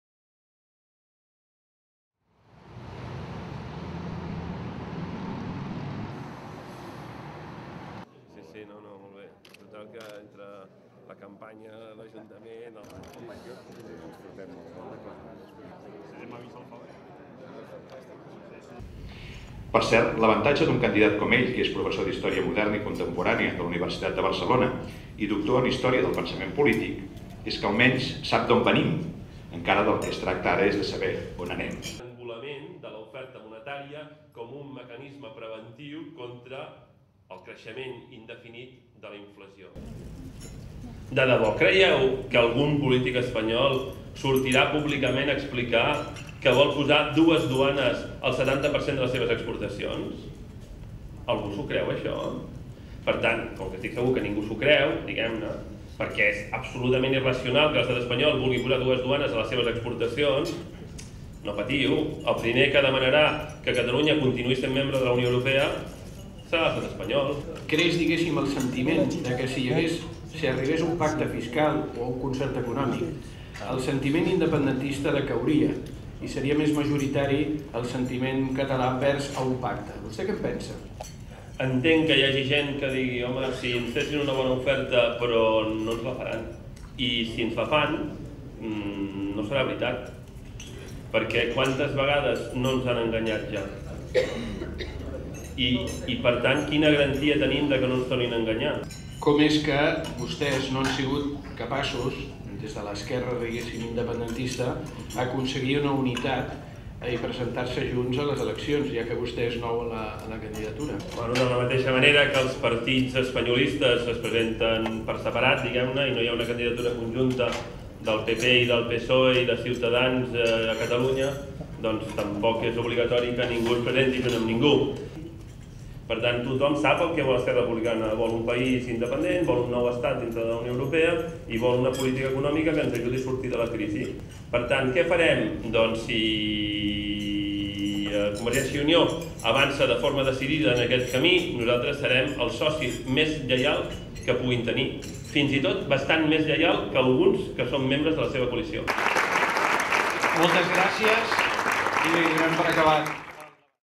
Vea la intervención de Oriol Junqueras en Barcelona Tribuna
El candidato de ERC a las elecciones catalanas ha asegurado que una Catalunya independiente "es urgente" y que continuaría dentro de la UE.

Raquel Quelart / Foto: Roser Vilallonga